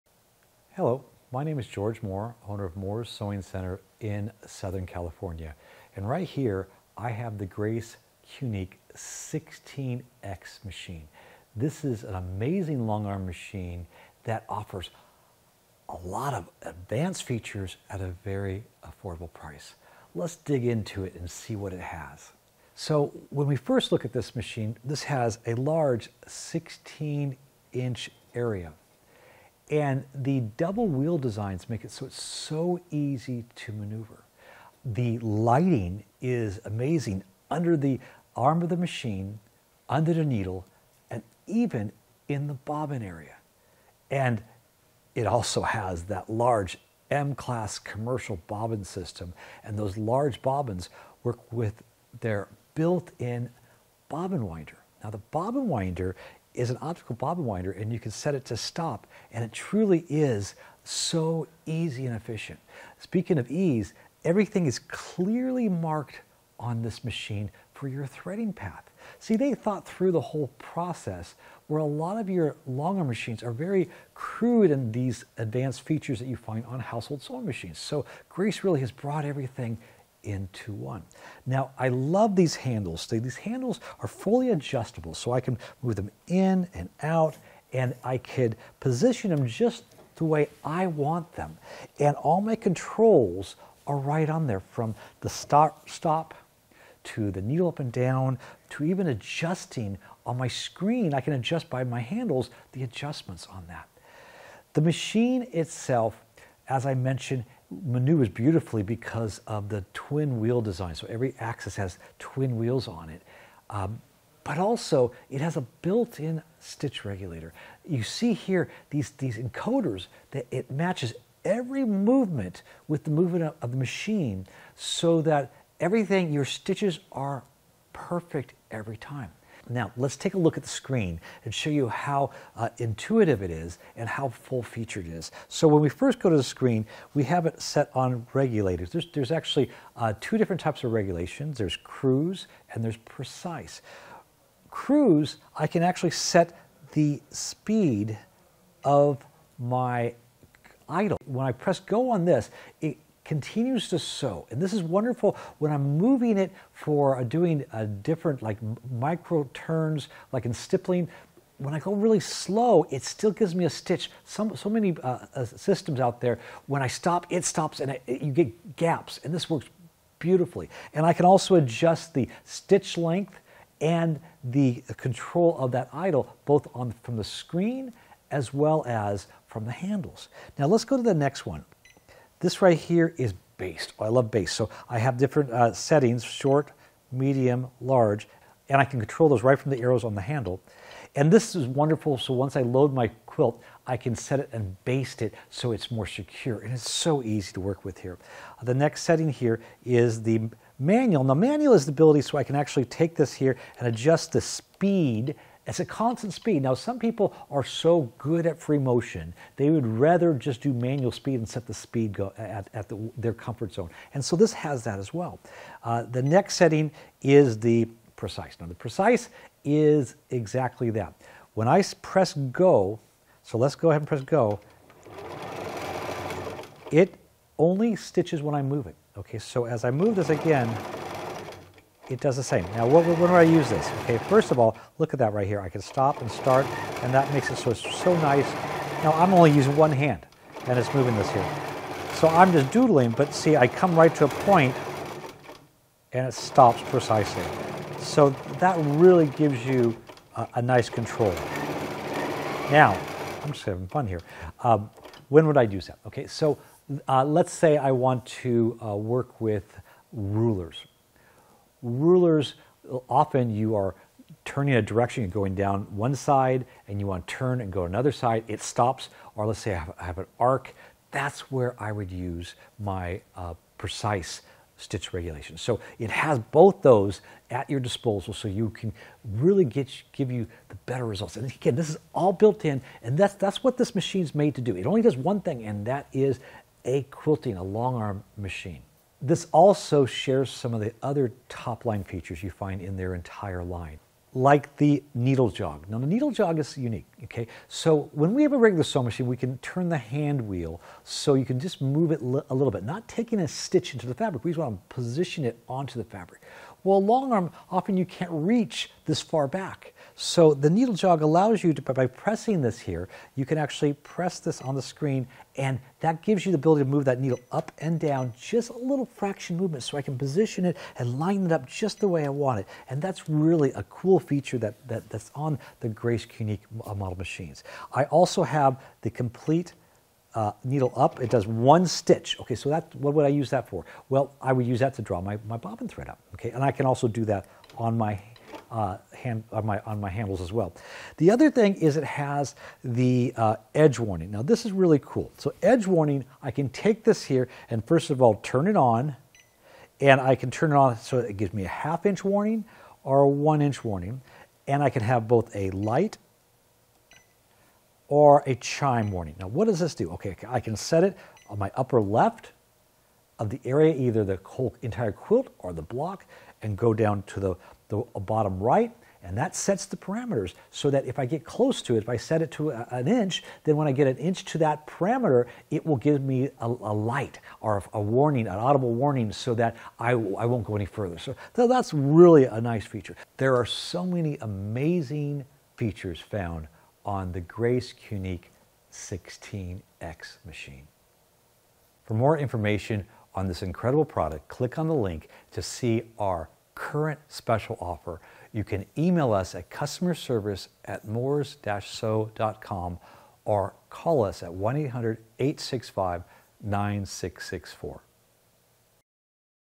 Grace Q'nique 16x Long Arm Machines | Moore's Sewing Center
George Moore describes the amazing features found on the Grace Q'nique 16x long arm machine.